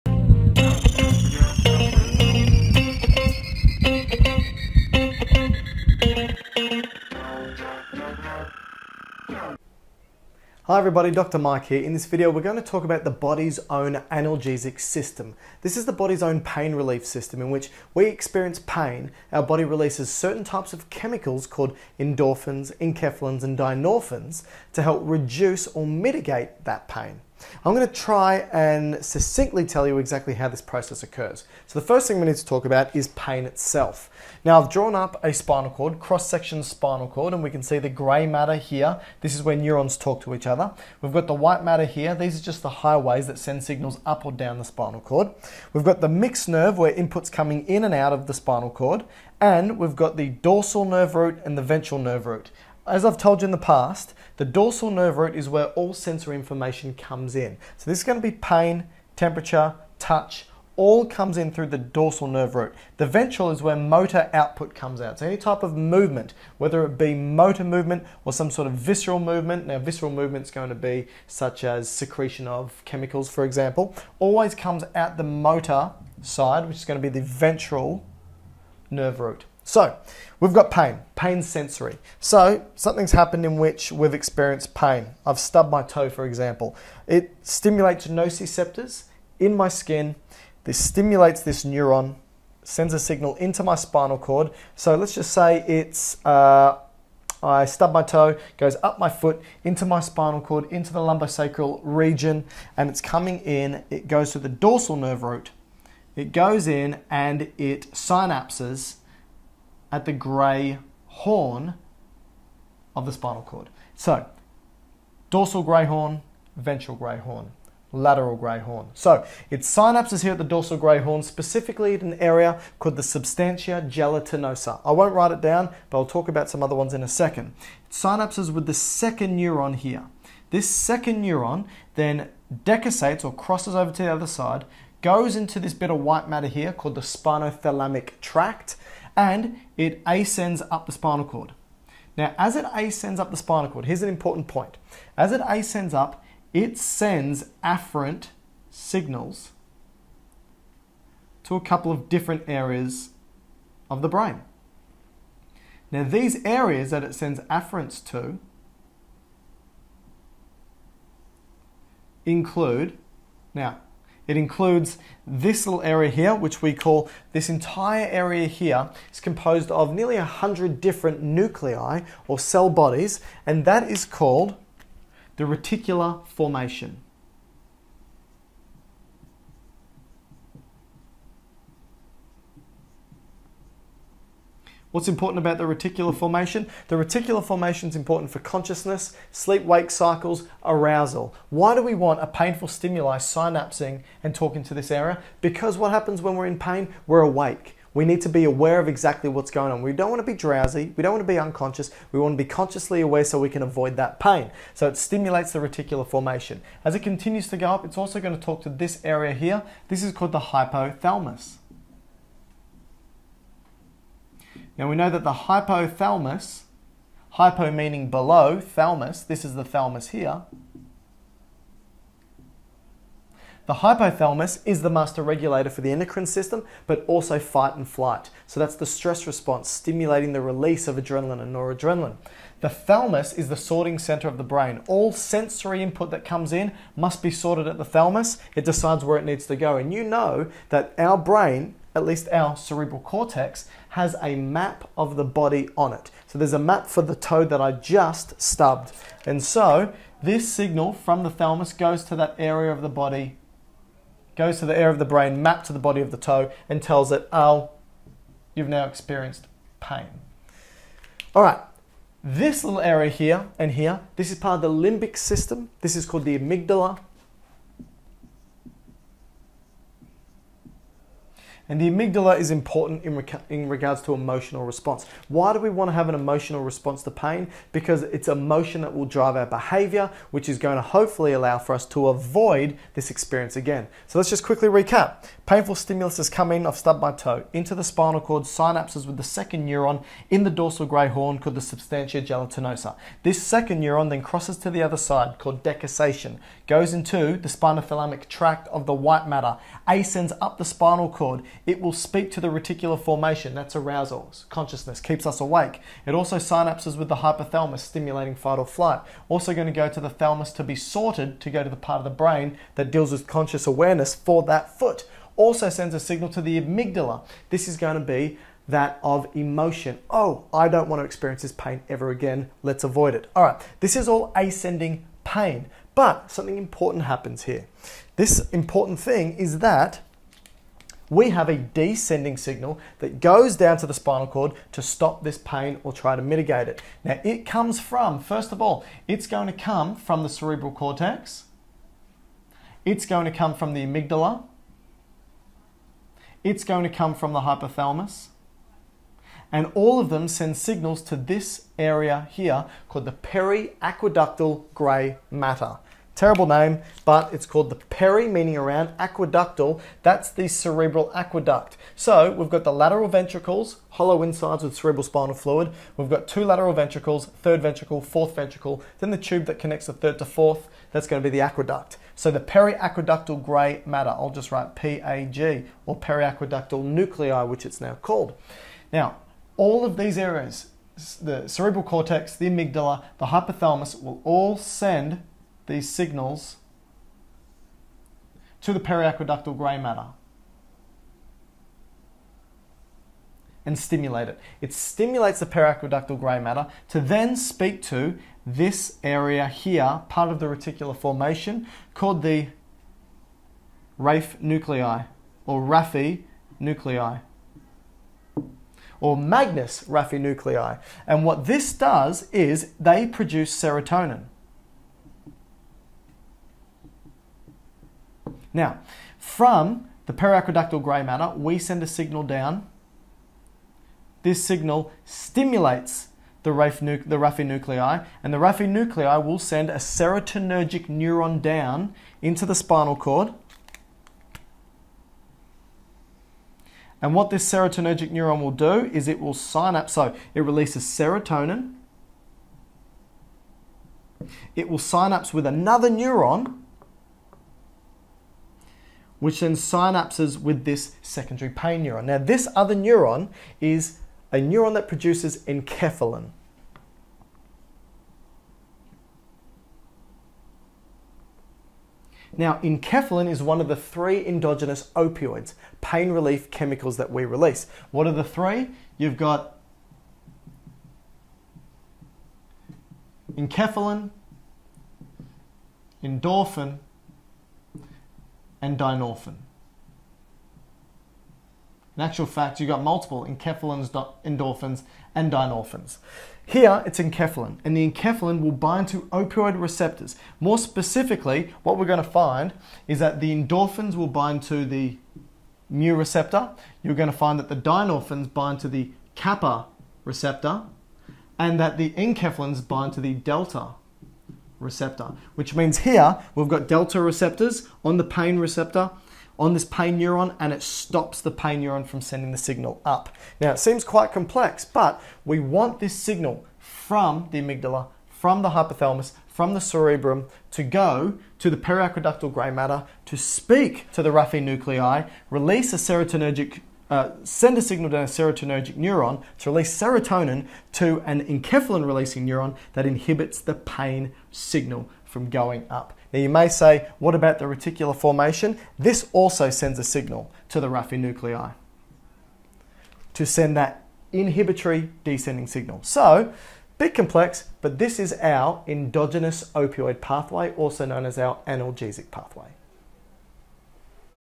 Analgesic Pathway | Endogenous Opioid System
The body has its own pain relief system that gets activated when experiencing pain.
As a painful stimulus ascends to the cerebral cortex (to become consciously aware of the pain) it sends signals to multiple other areas of the brain. This includes:
- Amygdala (emotion)
- Hypothalamus (flight/fight response)
- Reticular formation (arousal and consciousness)
1️⃣All of these regions, including the cortex, will send a signal to the
2️⃣Magnus Raphe Nucleus (located at the midline of the medulla) and will
3️⃣Send a serotonergic signal to the spinal cord
4️⃣Stimulate a neuron in the spinal cord that releases Enkaphalin (opioid)
5️⃣Enkephalin will bind to opioid receptors (delta opioid receptors) of both the pre- and post- synaptic pain neuron.
6️⃣Stopping the pain signal!!